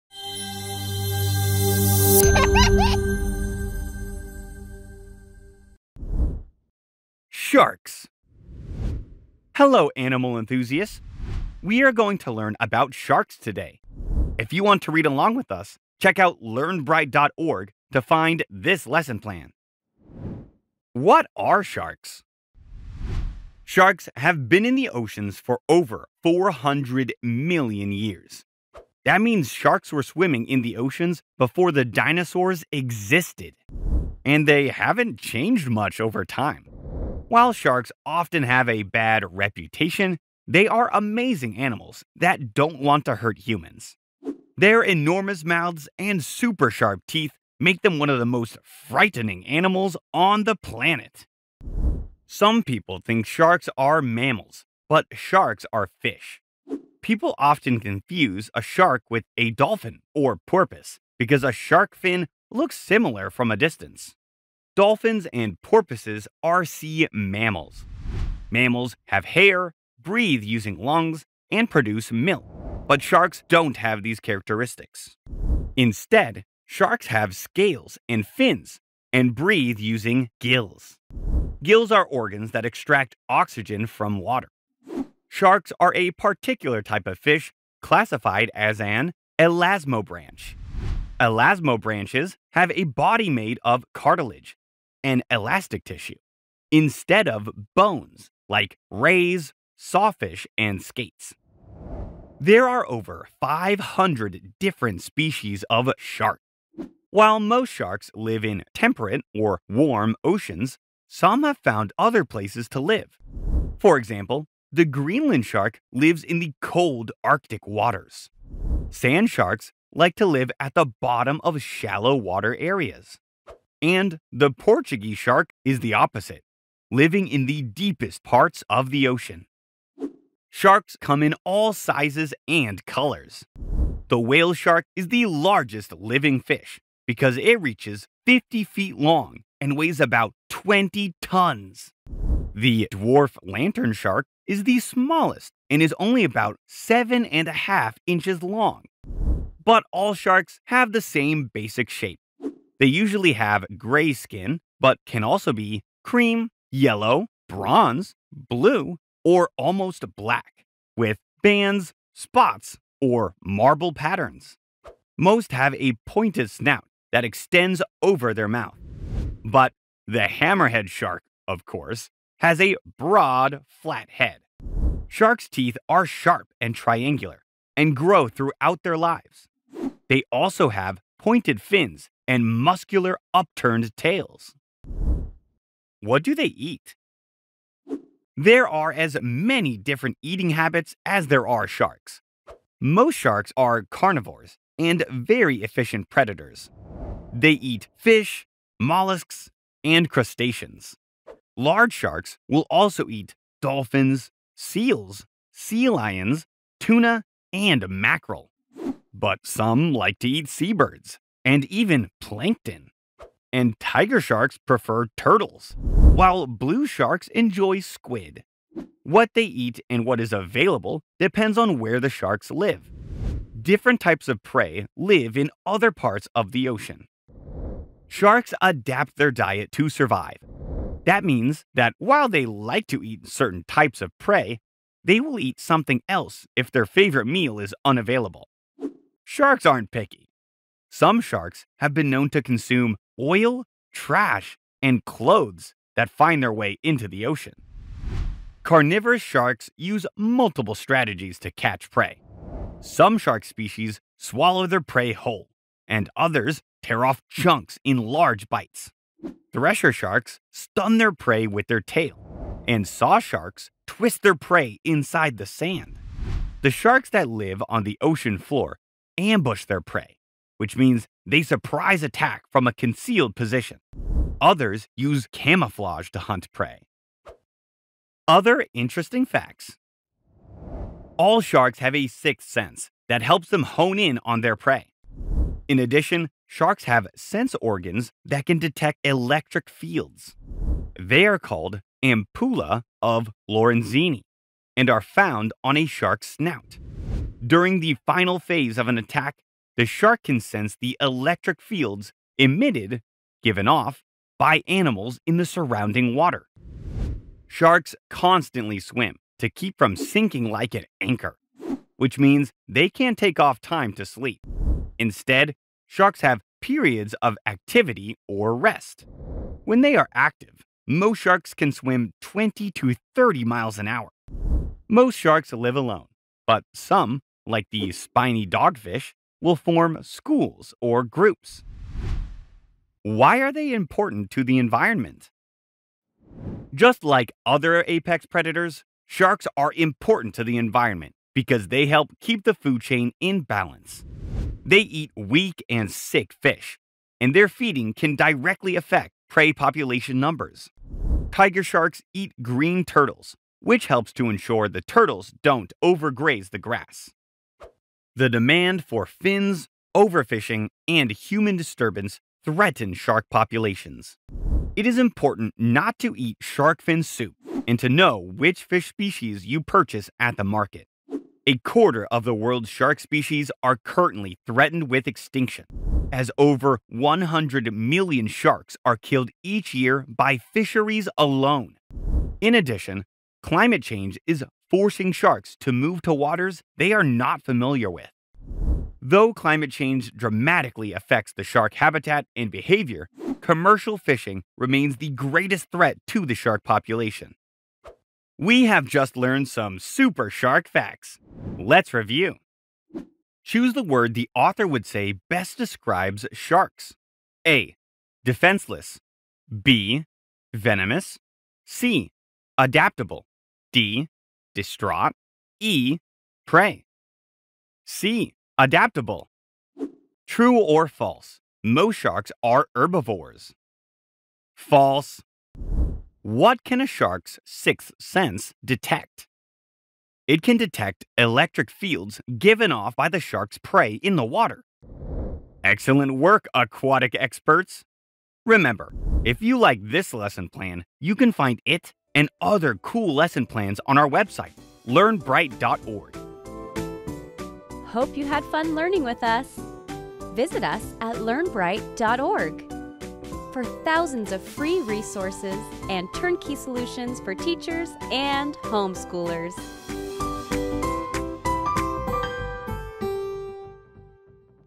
Sharks for Kids | Learn all about these big fish!
Did you know that sharks have been in the oceans for over 400 million years? That means sharks were swimming in the oceans before the dinosaurs existed. And they haven’t changed much over time. While sharks often have a bad reputation, they are amazing animals that don’t want to hurt humans. Their enormous mouths and super sharp teeth make them one of the most frightening animals on the planet.

There are as many different eating habits as there are sharks. Most sharks are carnivores and very efficient predators. They eat fish, mollusks, and crustaceans. Large sharks will also eat dolphins, seals, sea lions, tuna, and mackerel. But some like to eat seabirds and even plankton! And tiger sharks prefer turtles while blue sharks enjoy squid. What they eat and what is available depends on where the sharks live. Different types of prey live in other parts of the ocean.

The demand for fins, overfishing, and human disturbance threaten shark populations. It is important not to eat shark fin soup and to know which fish species you purchase at the market. A quarter of the world’s shark species are currently threatened with extinction. In addition, climate change is forcing sharks to move to waters they are not familiar with. Though climate change dramatically affects the shark habitat and behavior, commercial fishing remains the greatest threat to the shark population.

We hope you and your student(s) enjoyed learning about these fascinating fish! If you want to learn even more, head over to our website and download one of our many lesson plans about animals, full of activities, worksheets, and more!

What you will learn in Sharks for Kids:
0:00 Introduction
0:20 What are sharks?
1:42 Species of shark
2:10 Shark characteristics
3:05 Diet of a shark
4:38 Other interesting facts
5:32 Why are they important to the environment?
6:41 Review of the facts